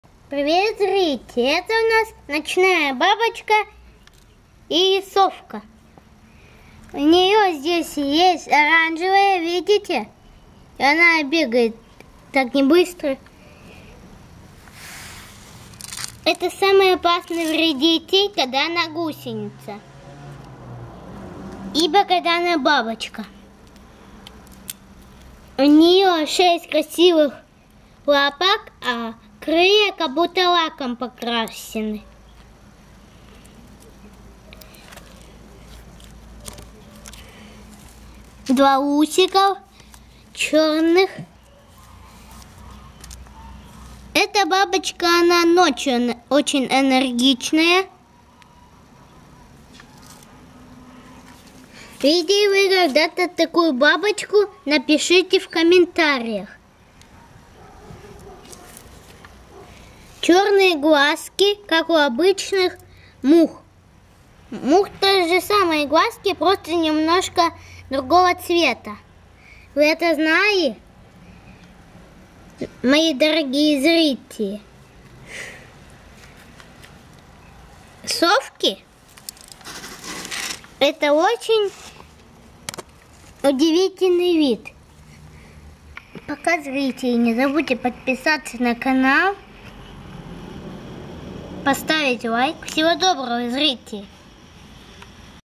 СОВКА - НОЧНАЯ БАБОЧКА. В Мире животных глазами ребенка. Никита Нюняев. Одесса Сентябрь 2017
Совки — необычные ночные бабочки. Сколько разновидностей совок существует в природе и как они выглядят? Об этом вам расскажут наши фото и описание насекомого.

Совки или ночницы – большое семейство чешуекрылых насекомых.  Насчитывается более 35 тысяч видов совок. В Европе обитает около 1800 видов, а в России более 1500 видов. Совки встречаются в различных уголках планеты. Для их жизни подходит любой климат. Совки отлично чувствуют себя в пустынях, горах и в условиях тундры.

Имеются крупные виды совок и совсем мелкие. У крупных видов размах крыльев может достигать 130 миллиметров, но встречаются и такие мелкие виды, которые имеют размах крыльев не более 10 миллиметров.